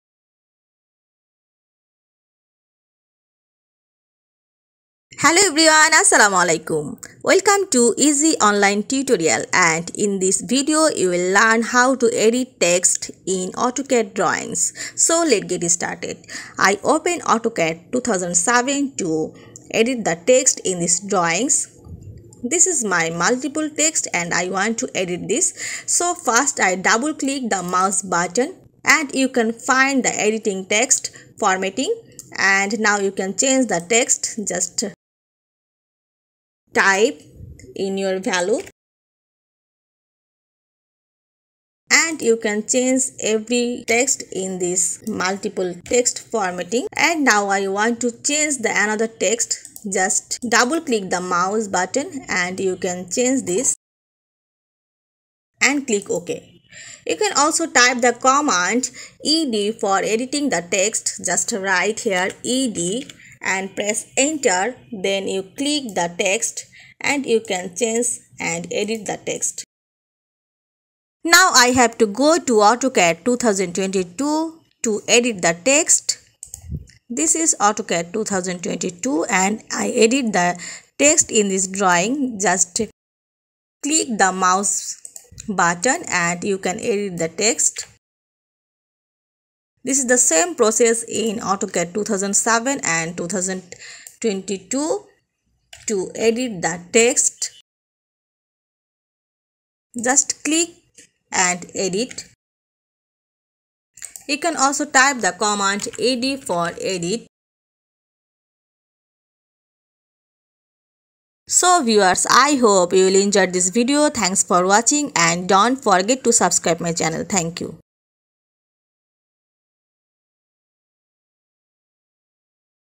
How to edit text in AutoCAD drawing
Hello everyone, Today I will show you, How to edit the text in AutoCAD drawing.